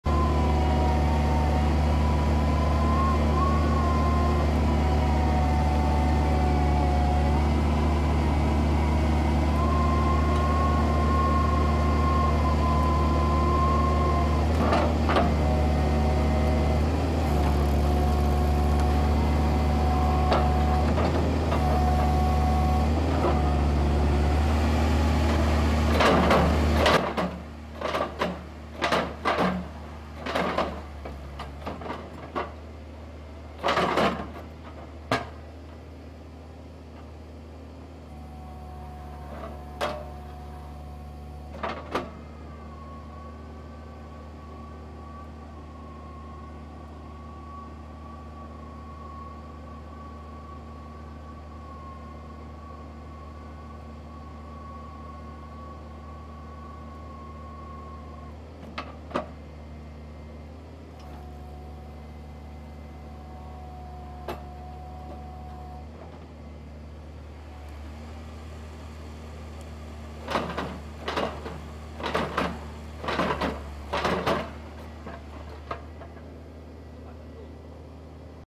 Tescar CF3 S while doing the test
Tescar CF3 S

TORQUE: 35 kNm
ENGINE POWER: 47,8kW
MAX DEPTH: 22 mts
MAX DIAMETER: 1.000 mm
TOTAL WEIGHT : 11 t

Hydraulic piling rig CF3 S

Provided with a 3 sections telescopic mast to work with a 22 m locking kelly bar, with the possibility to close down the 3 telescopic sections to reduce the total height and work in low headroom position even under 3500 m with a special "short" kelly bar for 10 m depth. With a max width of 2320 mm, the CF3 S can move in narrow places and restricted areas. The undercarriage is provided with rubber crawlers to work over fragile or luxury floors without problem.